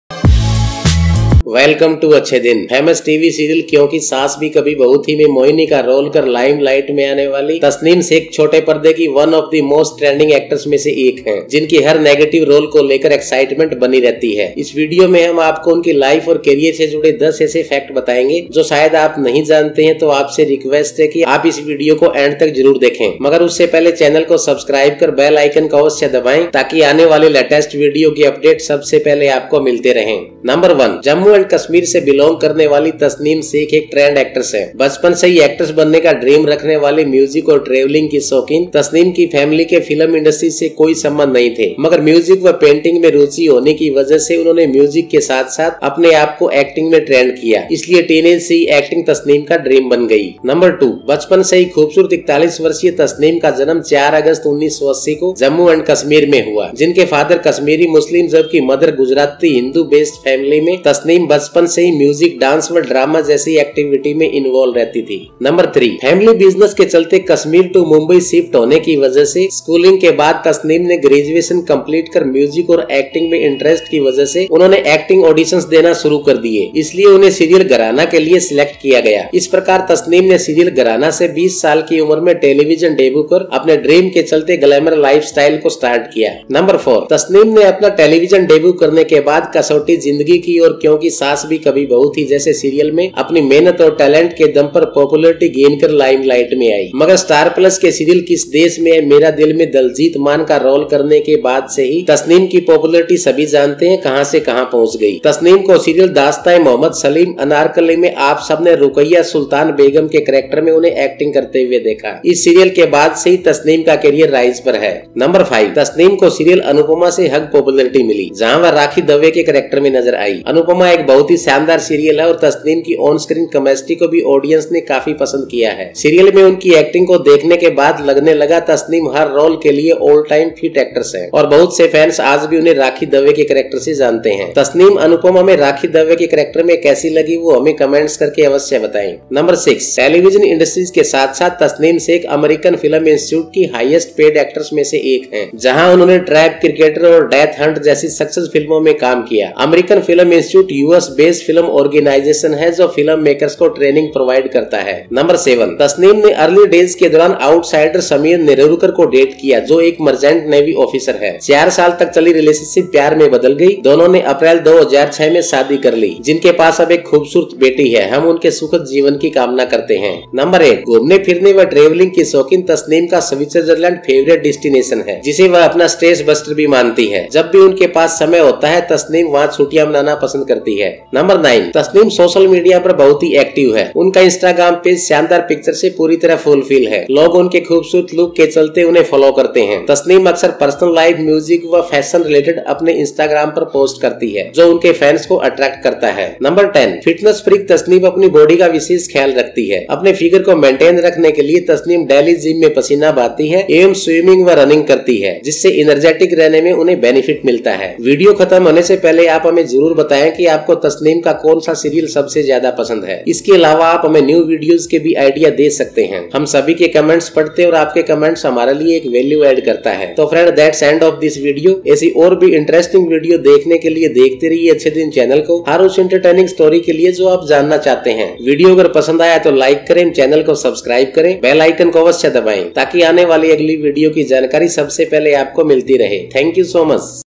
10 Facts You Didn't Know About Tassnim Sheikh | Anupamaa
Tasneem Sheikh, who plays Mohini in the famous TV serial "Kyunki Saas Bhi Kabhi Bahu Thi", is one of the most trending actresses of the small screen in the limelight, who have exquisite attention to every negative role! In this video, we will tell you 10 such facts related to his life and career, which you probably do not know, so request you to watch this video till the end!
Tassnim Sheikh - Tassnim Sheikh Facts - Tassnim Sheikh lifestyle -Tassnim Sheikh Biography -Tassnim Sheikh Anupamaa